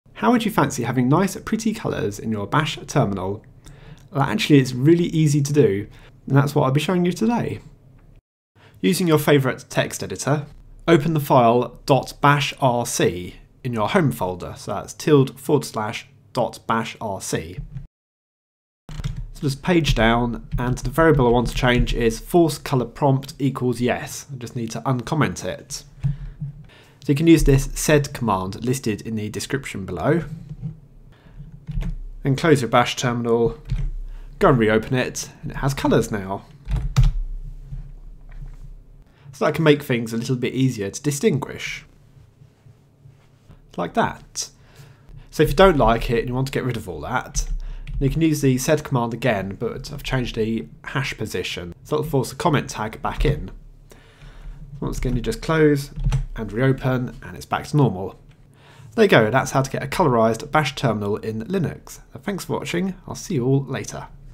Colored Bash Terminal
How to have a Coloured Bash Terminal in Linux. Works with any Linux Distribution, as long as you're using Bash as your Terminal. Also works through SSH connections.

Command:
sed -i 's/force_color_prompt=yes/force_color_prompt=yes/g' ~/.bashrc
Then close and re-open your Bash terminal